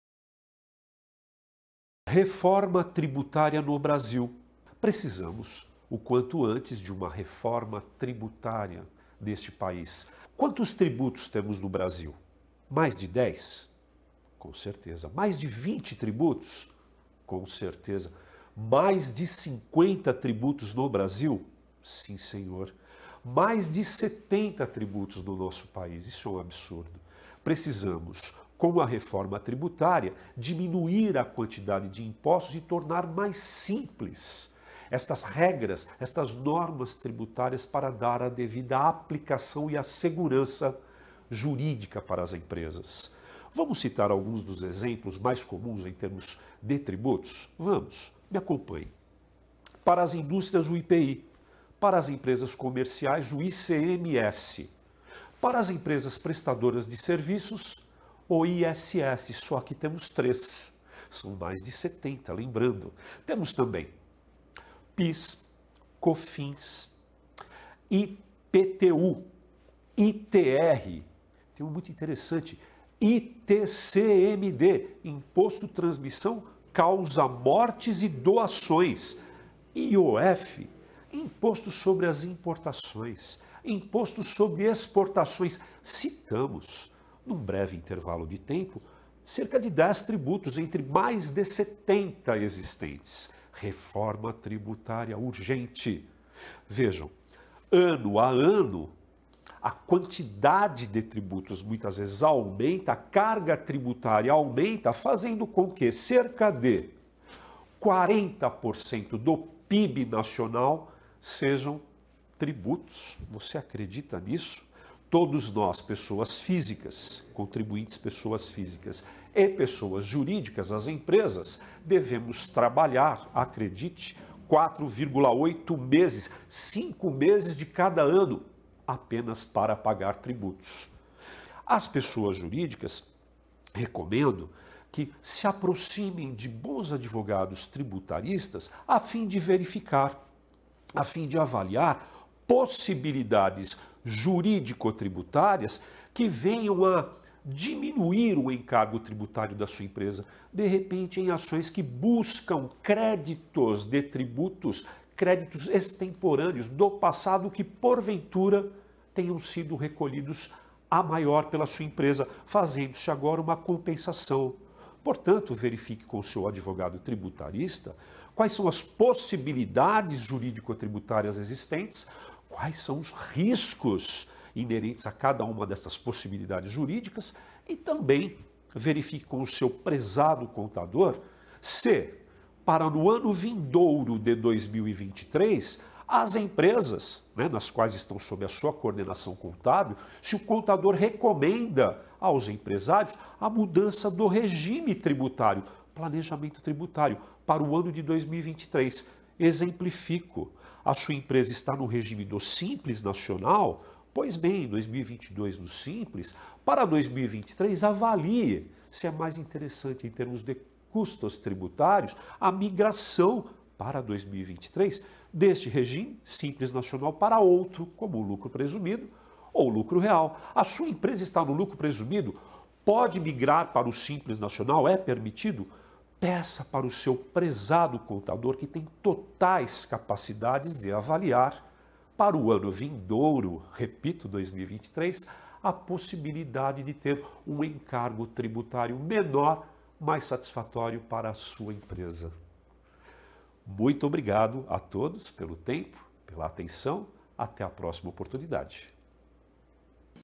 Precisamos de uma reforma tributária
“Precisamos o quanto antes de uma reforma tributária neste país. Precisamos diminuir a quantidade de impostos e tornar mais simples essas normas tributárias, para dar a devida aplicação e a segurança jurídica para as empresas”, afirma enfaticamente o auditor e perito contador, Vitor Stankevicius. Segundo Vitor, o Brasil tem hoje mais de 70 tributos como o IPI para as indústrias, o ISS para empresas prestadoras de serviços, imposto sobre importações e exportações, entre outros. “Vejam, ano a ano, a carga tributária aumenta, fazendo com que cerca de 40% do PIB nacional sejam tributos. Você acredita nisso?”, questiona. Vitor recomenda que empresários se aproximem de bons advogados tributaristas para avaliar possibilidades jurídico tributárias de diminuir o encargo tributário das suas empresas. “De repente, em ações que buscam créditos de tributos, créditos extemporâneos do passado que porventura tenham sido recolhidos à maior pela sua empresa, fazendo se agora uma compensação”. Vitor ainda recomenda que os empresários verifiquem com seus contadores se para o ano de 2023 há a orientação de mudança no regime tributário. “Para 2023, avalie se é mais interessante em termos de custos tributários a migração para outro regime”, conclui.
SINDICATO DA MICRO E PEQUENA INDUSTRIA DE RONDÔNIA